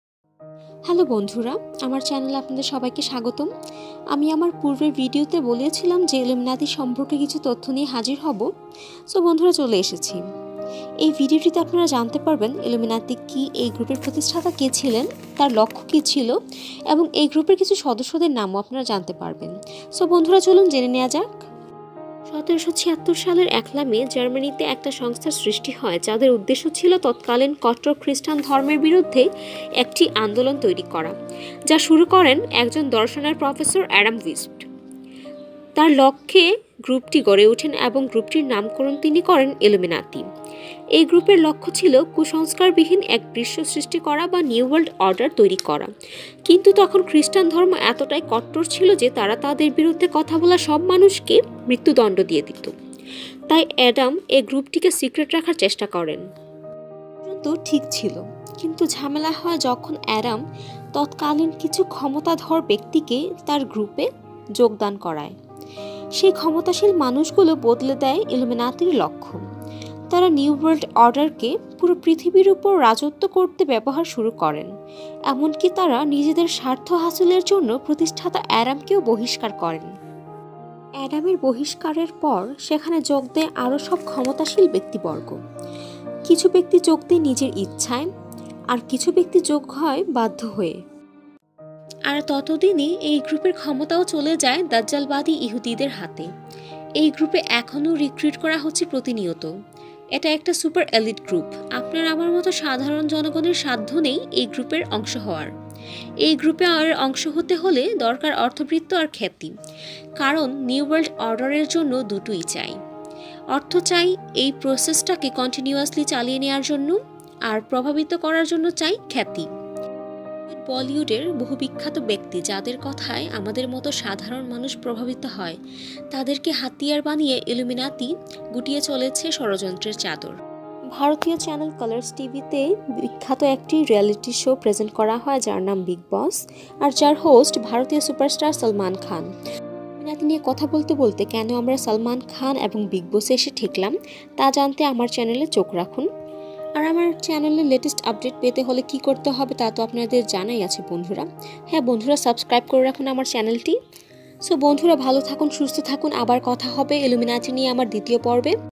বিগ বস হাউজ, সালমান খান ইলুমিনাতির সদস্য |salman khan member of secret society
কিছু প্রশ্ন এবং তার উত্তর যেমন

What is illuminati? who is the founder of illuminati?
বড় বড় সব অভিনেতা অভিনেত্রী এই গ্রুপ থেকে মোটা অংকের টাকা পায়।
ইলুমিনাতি নামের সিক্রেট সোসাইটি সম্পর্কে জানতে পুরো ভিডিওটি দেখুন।